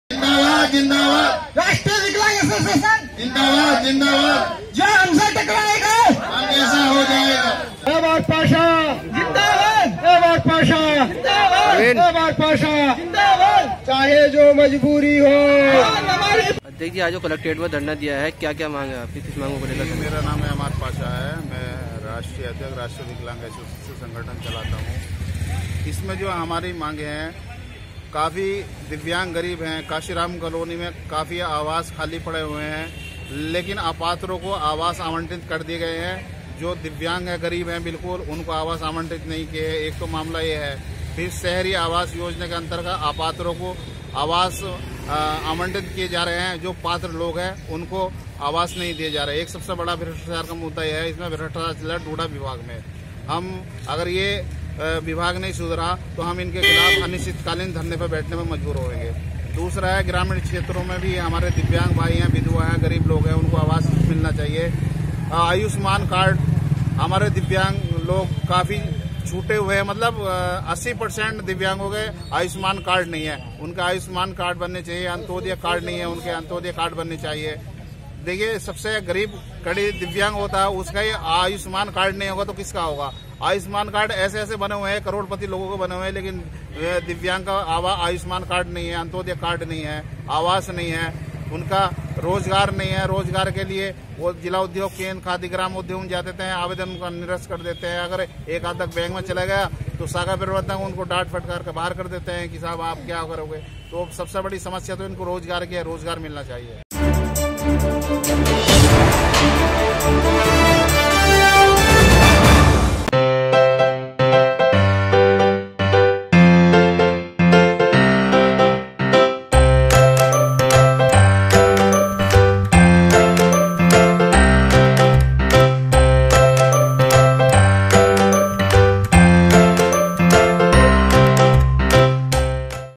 विभिन्न मांगों को लेकर दिव्यांगों ने कलेक्ट्रेट में किया प्रदर्शन
. अपनी मांगों को लेकर दिव्यांगों ने किया प्रदर्शन
. जिला प्रशासन पर लगाया मांगों को पूरा न करने का आरोप
. जिला प्रशासन के खिलाफ आंदोलन करने की दी धमकी